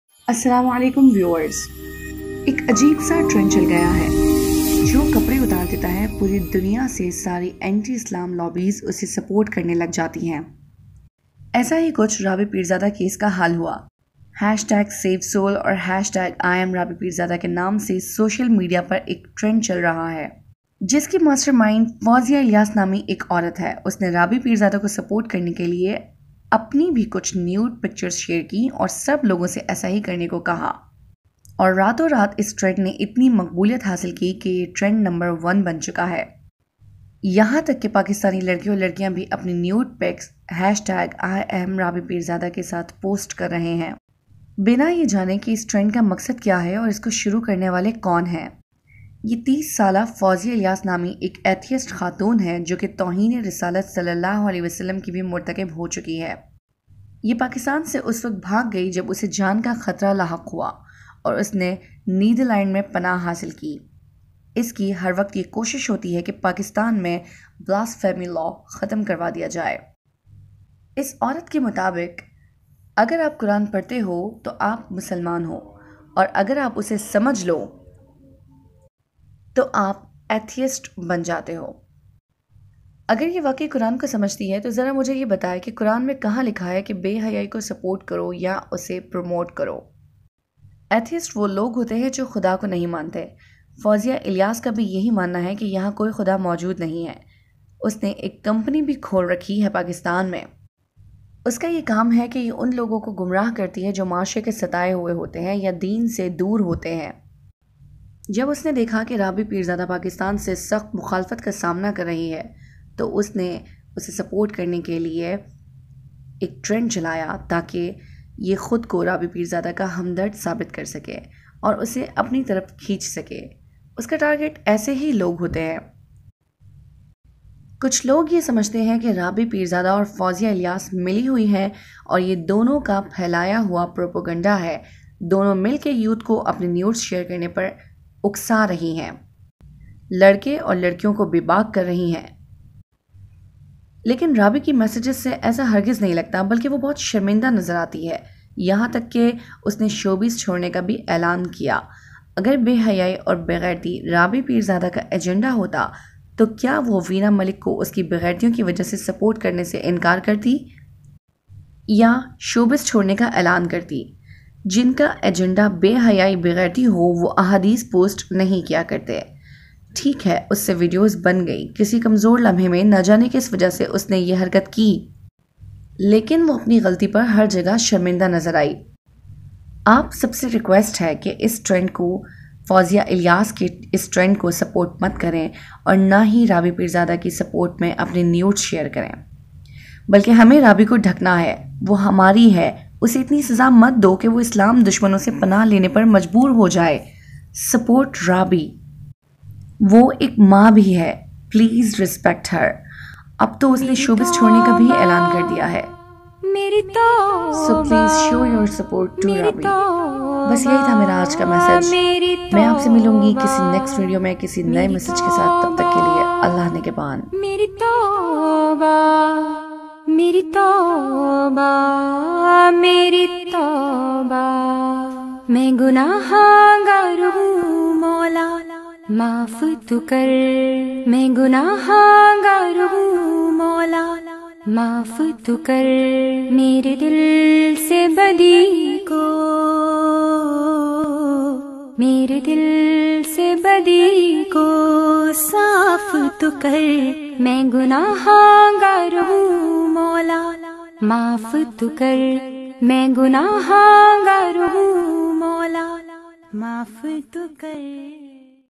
Rabi pirzada and fouzia ilyas | Story of imrabipirzada trend
Rabi pirzada and fouzia ilyas connection
Story of imrabipirzada, trend...
گلوکارہ رابی پیرزادہ کی مشکلات میں مزیداضافہ ہوگیا ہے کیونکہ ایف آئی اے کی جانب سے غیر اخلاقی تصاویر اور ویڈیوزہٹانے سےانکار کردیا گیا۔

رابی پیرزادہ کی غیر اخلاقی ویڈیو و تصاویرسے متعلق تنازع تاحال حل نہ ہوسکا۔ گلوکارہ کی جانب سے ایف ائی اے کودرخواست دائر کی گئی ہے جس میں ویڈیواور تصاویر فوری طور پرسوشل میڈیا ویب سائٹس سے ہٹانے کی استدعا کی گئی ہے۔گلوکارہ کی جانب سے موقف اختیار کیا گیا ہے کہ انہیں اور ان کے خاندان کوتنازع سے متعلق شدید ذہنی اذیت کاسامناہے۔رابی پیرزادہ نےدرخواست میں موقف اختیارکیاہے کہ انہوں نے کمپنی کواپنا فون واپس کیا تھا جس میں ان کی وڈیوزاور تصاویر موجود تھیں اور سوشل میڈیاپر وائرل ہوگئیں تھیں۔

ایف آئی اے نےرابی پیرزادہ کی تصاویراورویڈیوزکوسوشل میڈیاویب سائٹس سےہٹانے کے حوالے سےکہا گیا ہےکہ یہ کام ان کا نہیں بلکہ پاکستان ٹیلی کمیونیکیشن اتھارٹی (پی ٹی اے) کی ڈیوٹی ہےاور اس سلسلےمیں گلوکارہ کی درخواست پی ٹی اے کوارسال کردی گئی ہے۔